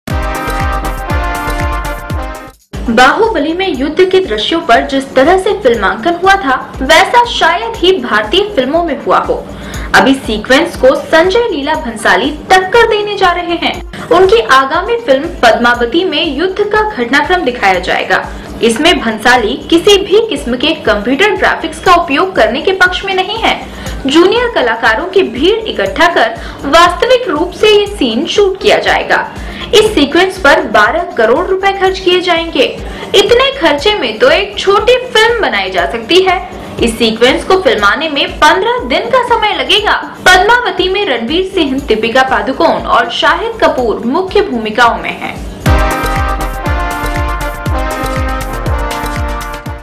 बाहुबली को टक्कर देगा 12 करोड़ का यह सीन
बाहुबली में युद्ध के दृश्यों का जिस तरह से फिल्मांकन हुआ था वैसा शायद ही भारतीय फिल्मों में हुआ हो। अब इस सीक्वेंस को संजय लीला भंसाली टक्कर देने जा रहे हैं। उनकी आगामी फिल्म 'पद्मावती' में युद्ध जूनियर कलाकारों की भीड़ इकट्ठा कर वास्तविक रूप से यह सीन शूट किया जाएगा। इस सीक्वेंस पर 12 करोड़ रुपये खर्च किए जाएंगे।